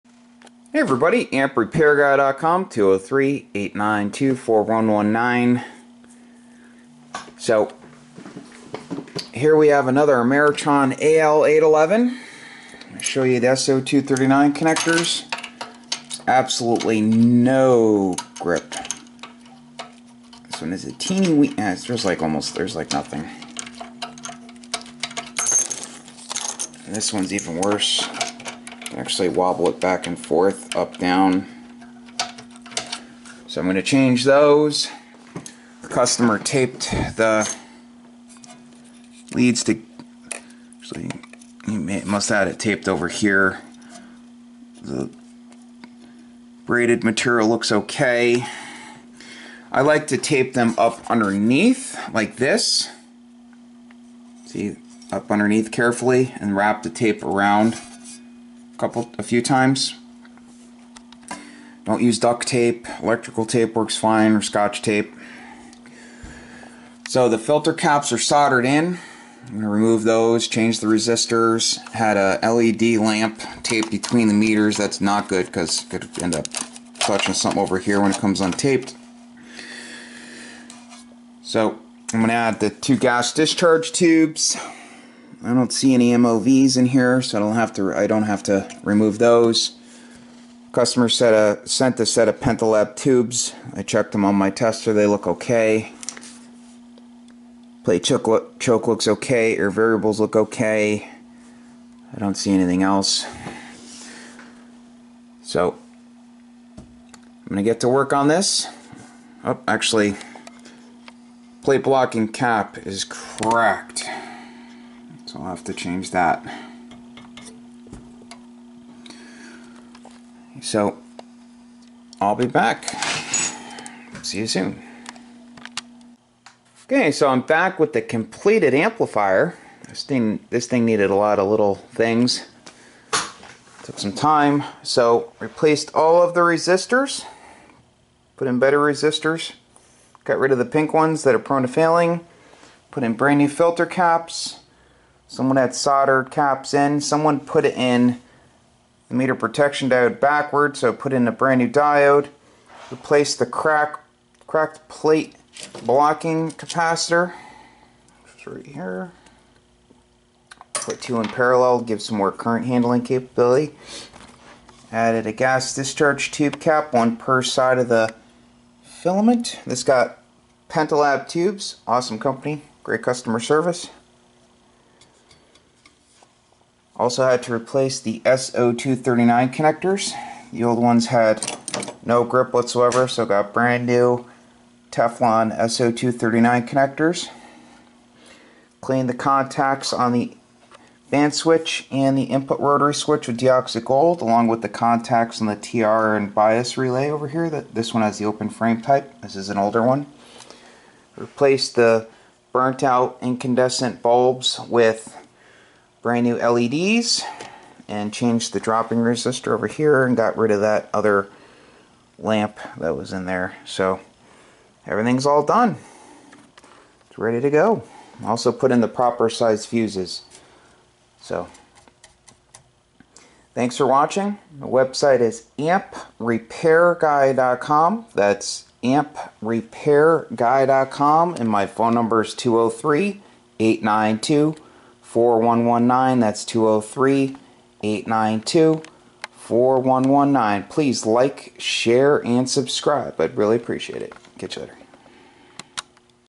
Ameritron Al811 Repairs And Modifications
Here I show the repair and modification of an Ameritron al811